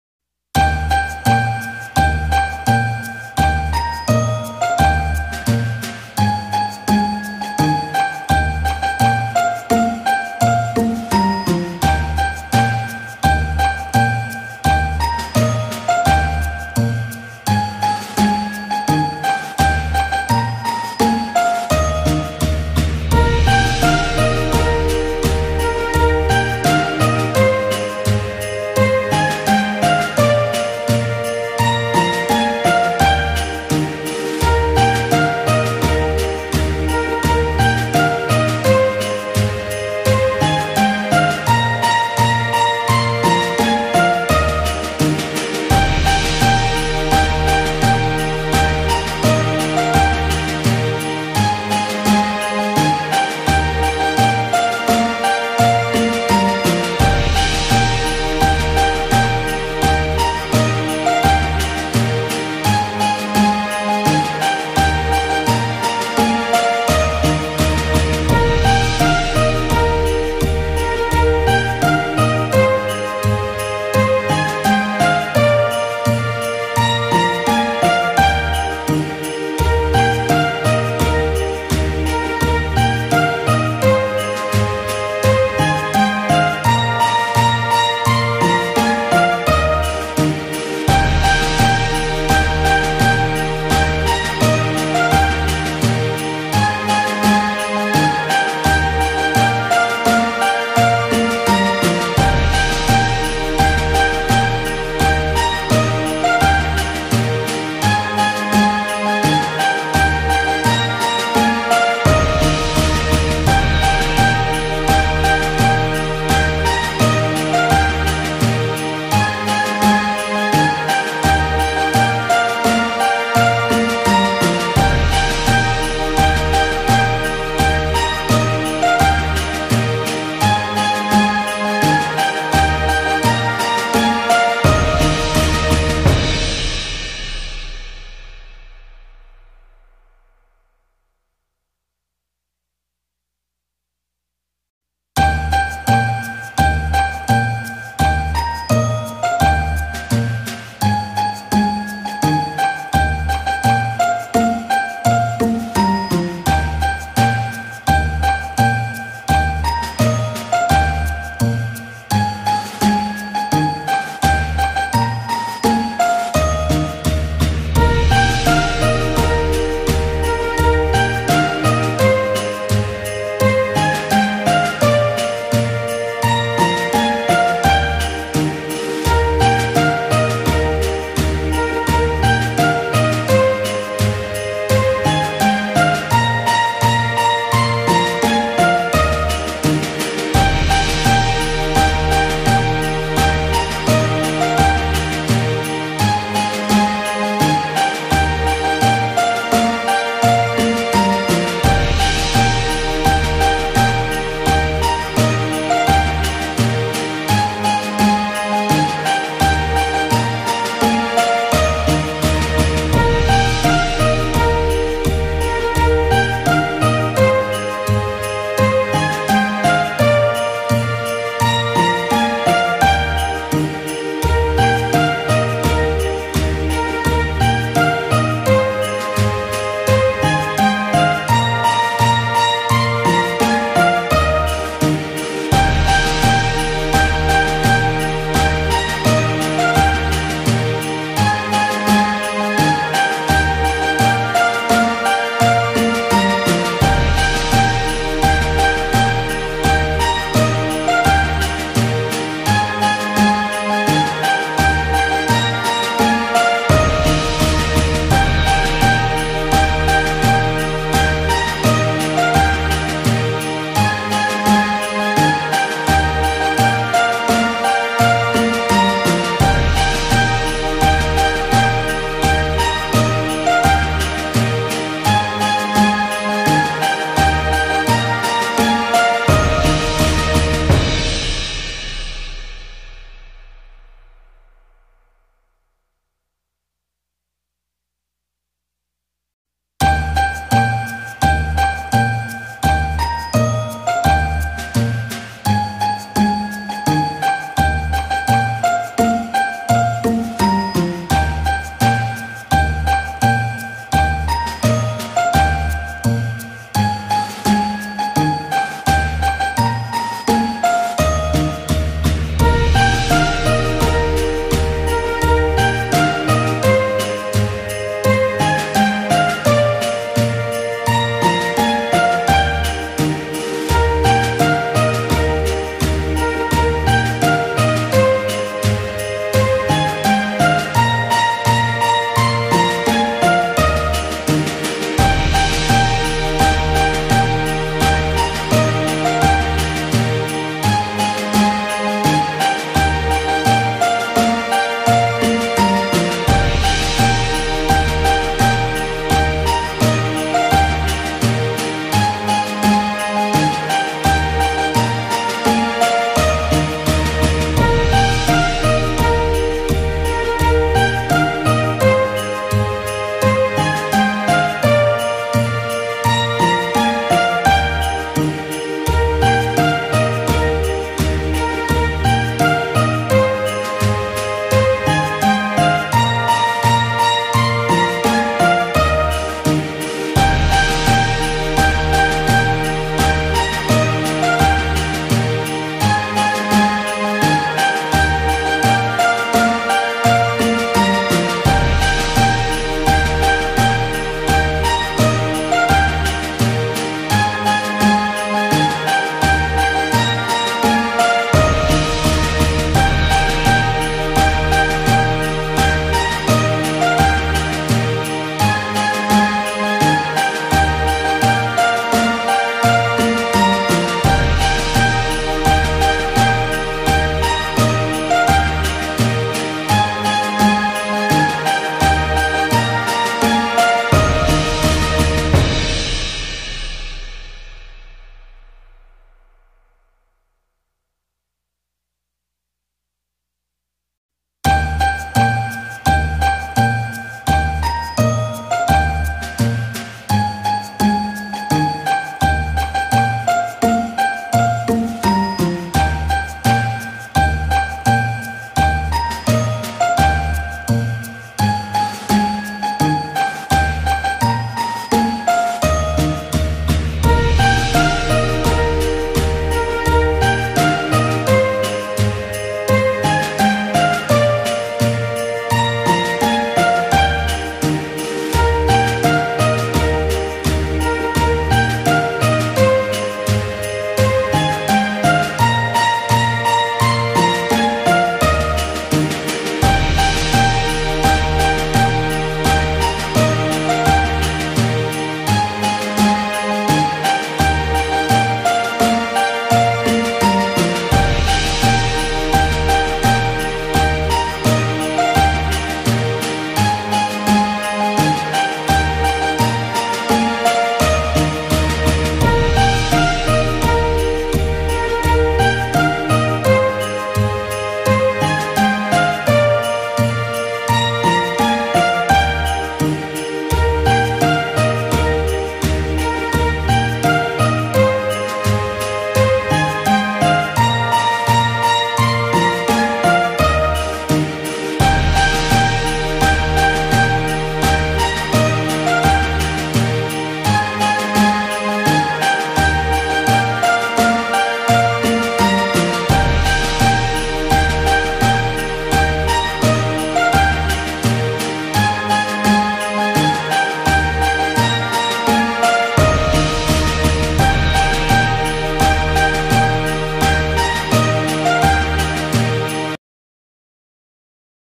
Christmas Decoration Ideas 2024 | DIY Christmas Trees | Christmas Crafts
This video guides to create 13 different types of Christmas trees at home for this Christmas. Quick and easy way to decorate home and diy for kids.